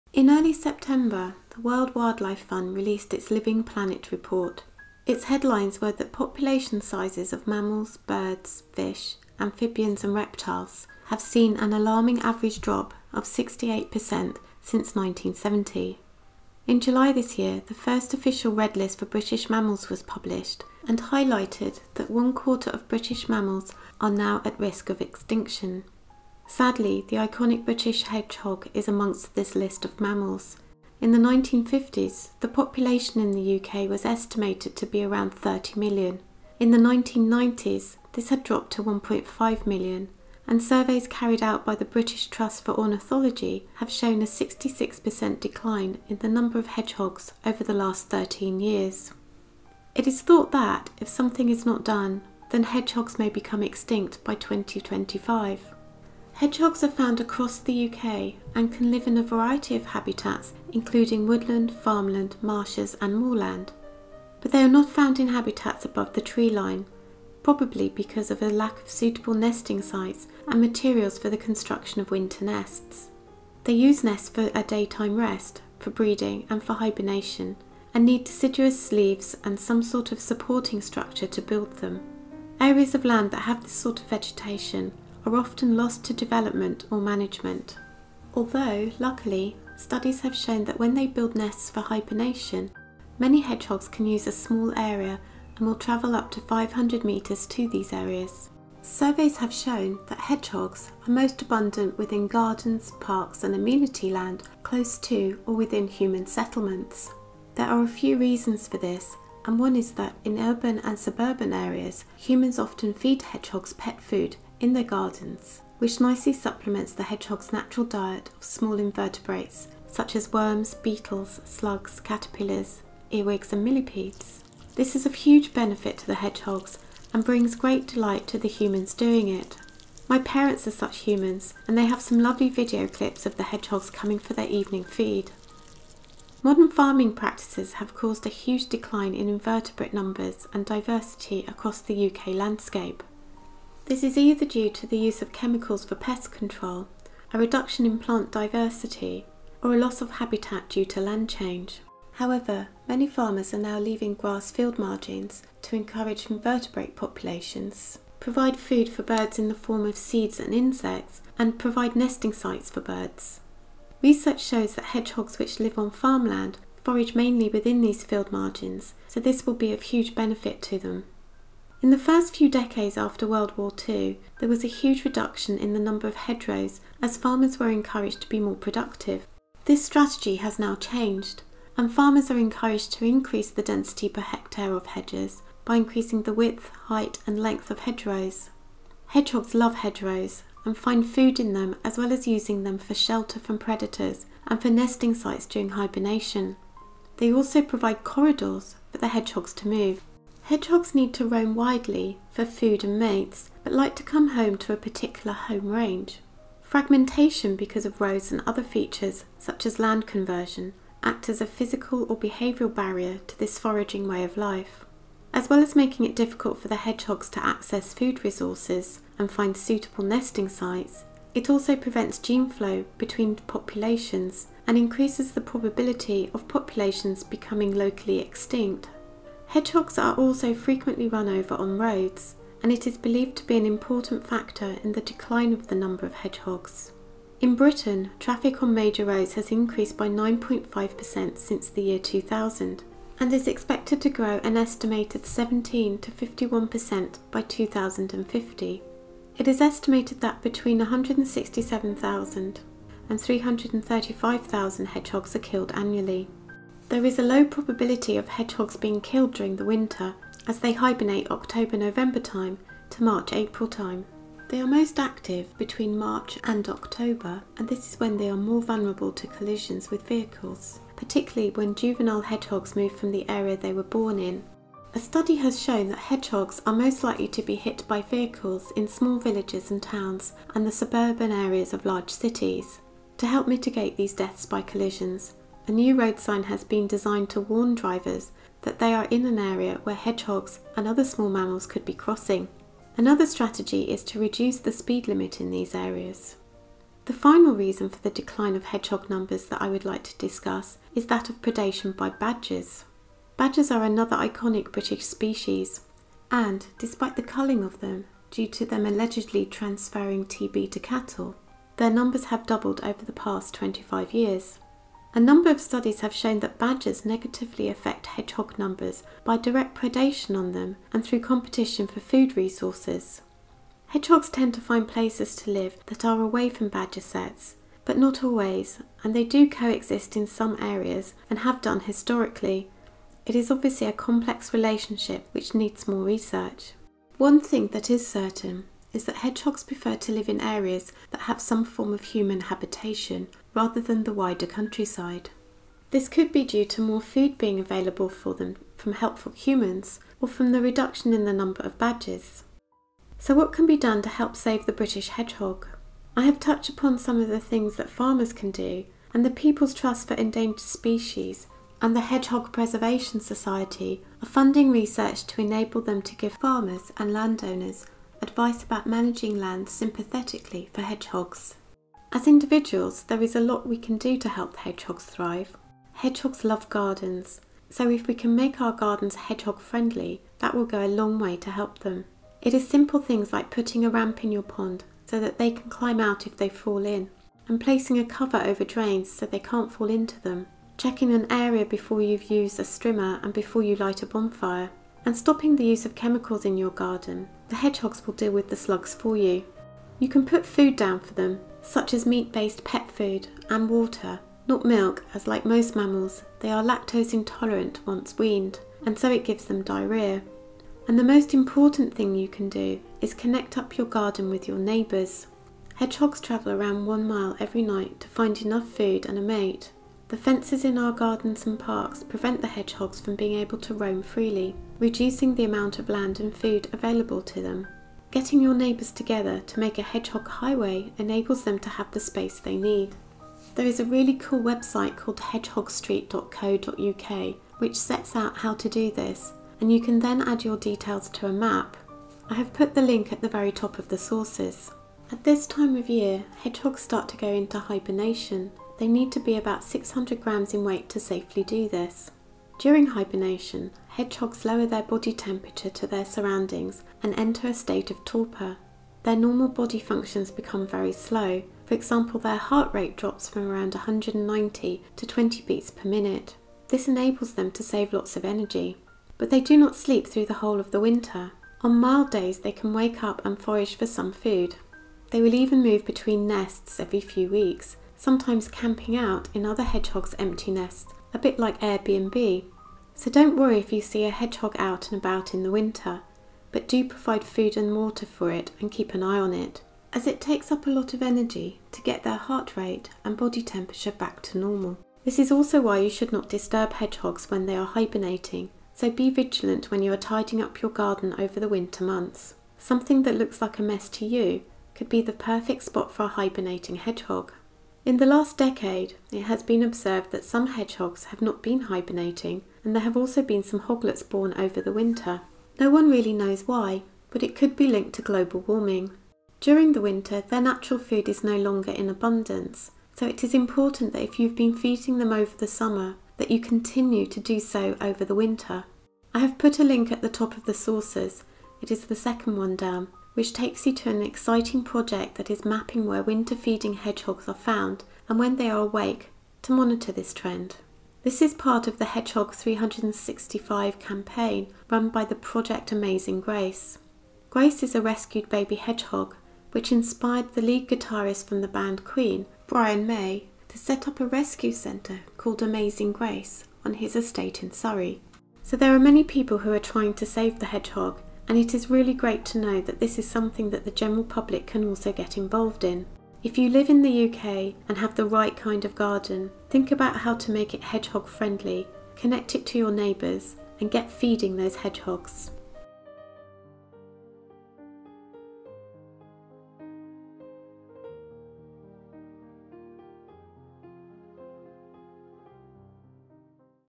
Living On The Hedge: UK Hedgehogs Vulnerable To Extinction
In 2020 the IUCN identified the iconic British Hedgehog as being vulnerable to extinction.  This video explores why this is happening and what we, the British public can do about it.